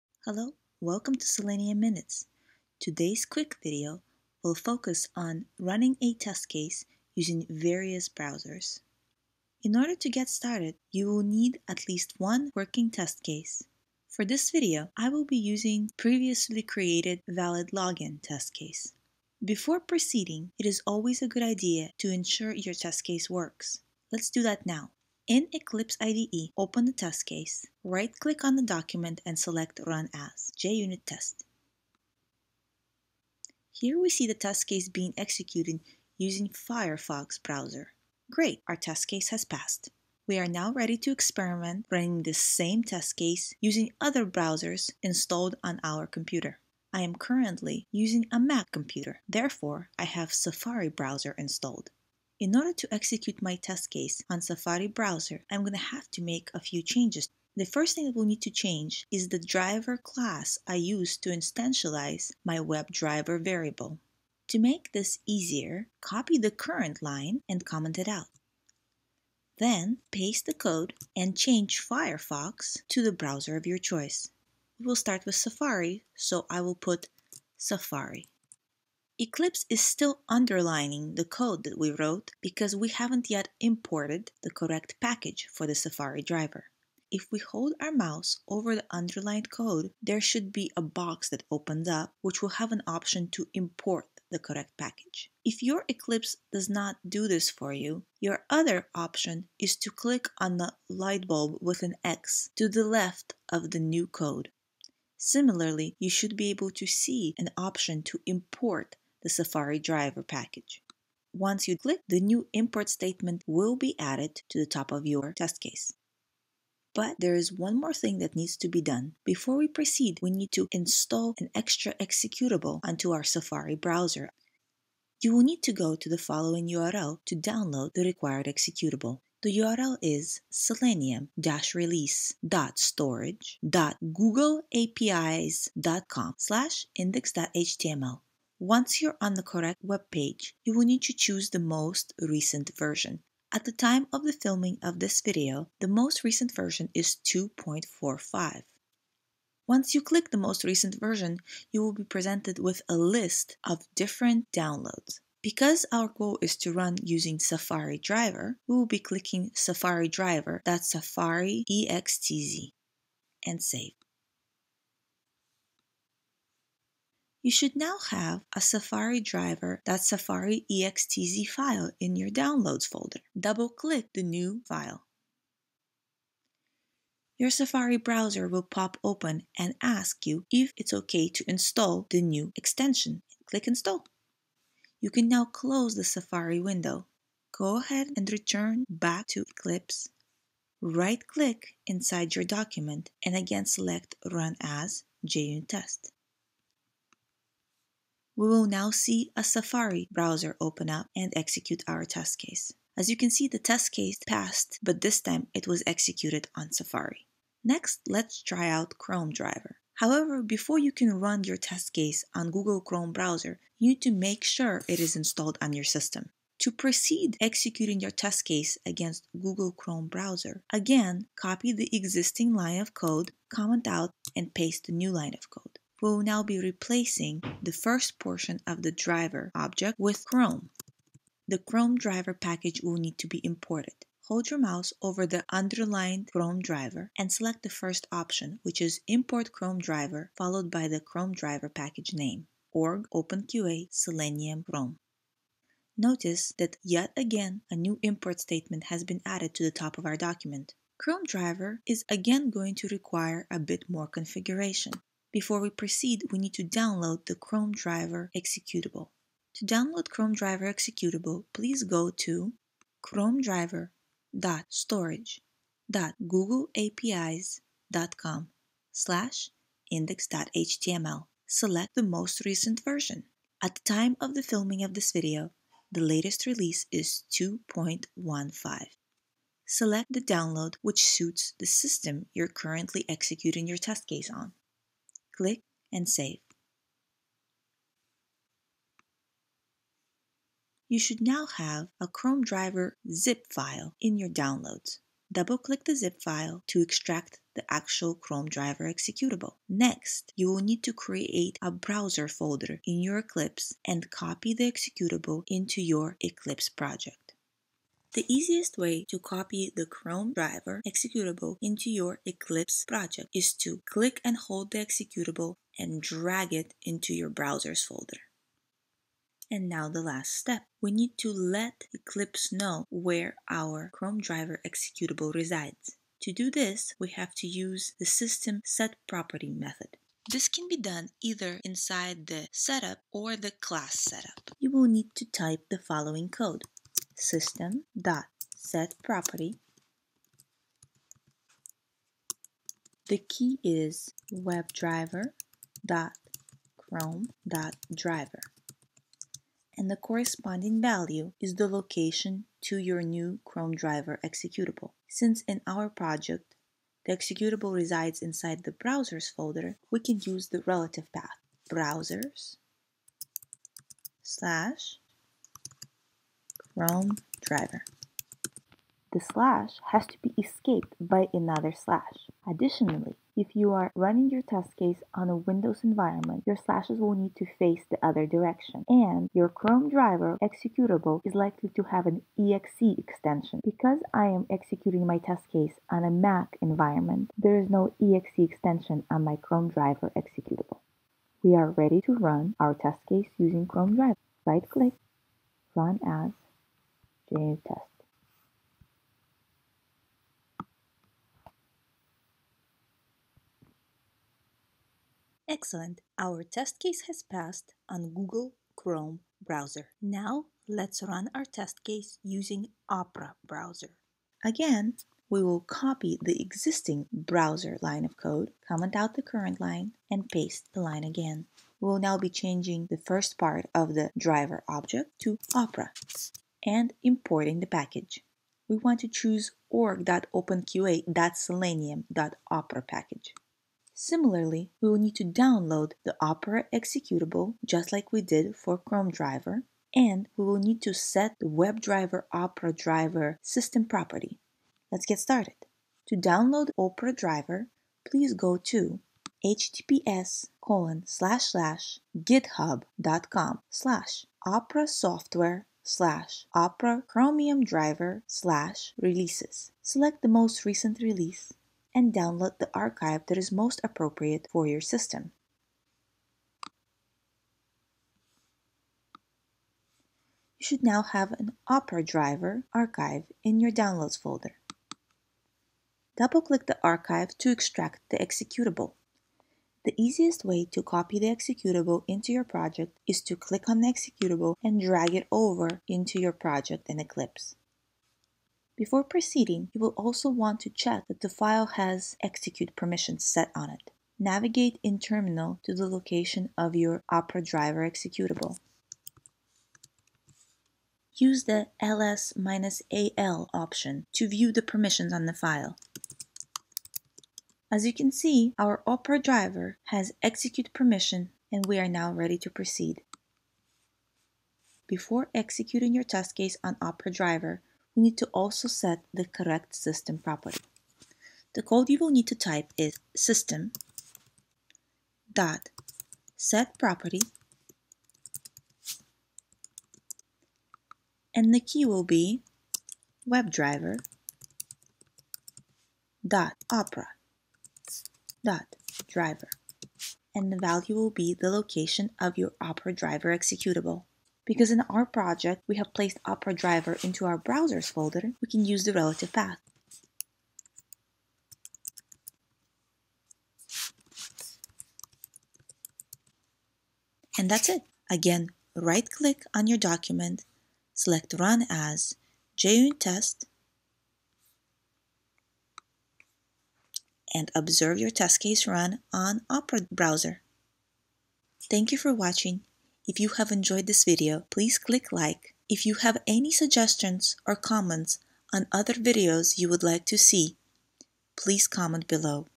Selenium WebDriver Java Test Automation using Google Chrome, Opera, and Safari browser
In less than 10 minutes, configure you existing test case to execute on 4 different browsers! Run your test on Google Chrome, Safari, Opera, and Firefox.